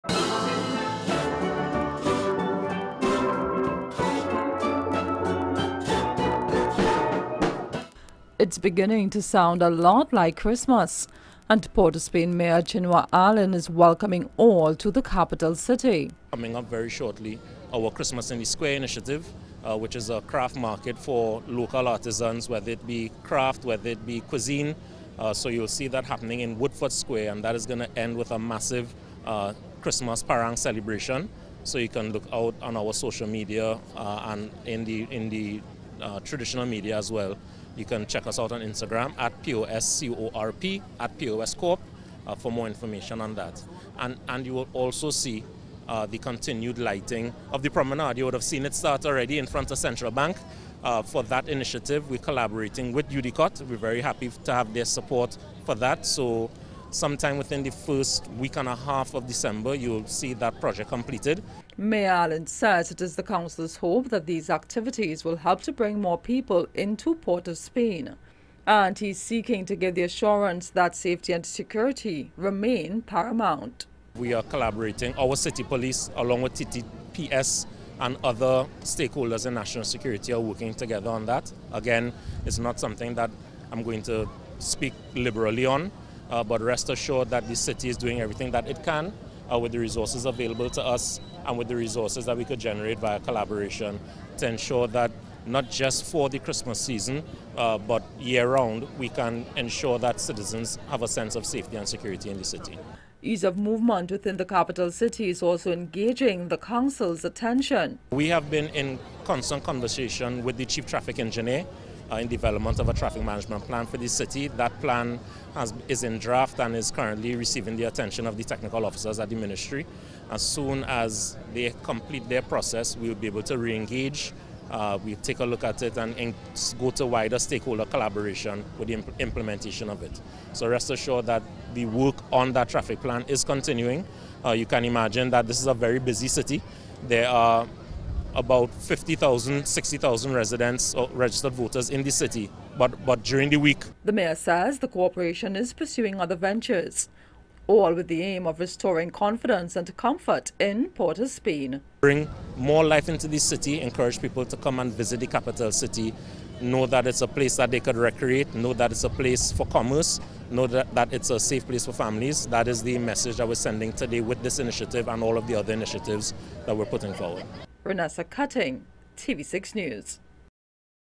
POS MAYOR WELCOMES ALL TO THE CITY THIS XMAS
Port of Spain Mayor Chinua Alleyne is welcoming everyone to the capital city.. for the Christmas season and says the Corporation will be putting things in place for the year-round safety and comfort of all to enjoy being downtown.